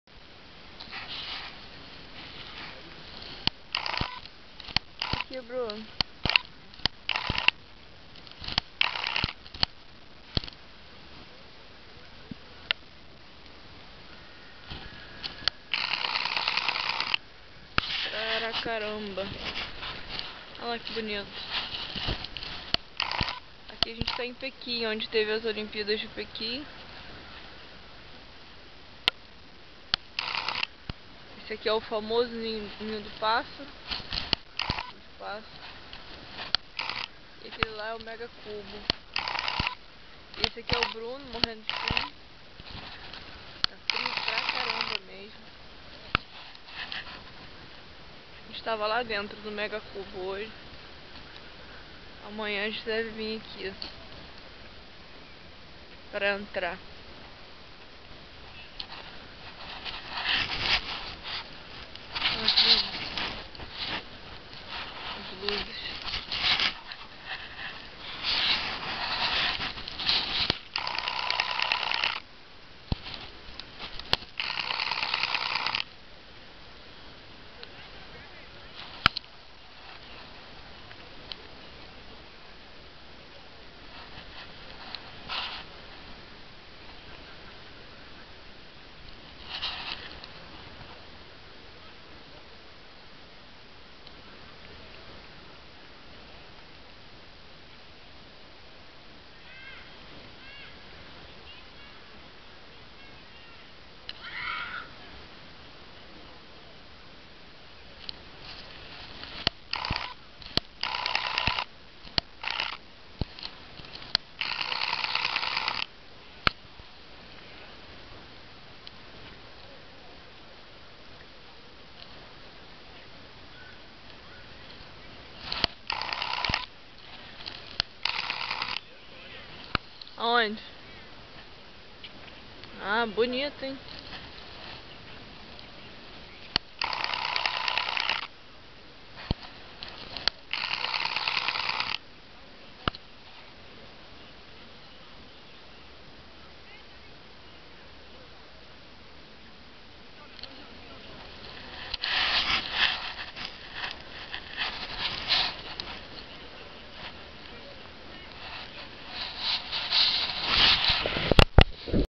Parque Olímpico em Pequim a Noite - Viagem Mundial
O parque olímpico em Pequim é um espaço bem grande onde você encontra as estruturas olímpicas como o famoso estádio nacional conhecido como ninho de pássaro e também o lindo Cubo d'Água e a tocha olímpica, além de outras estruturas.

O local tem fácil e rápido acesso através das linhas de metrô.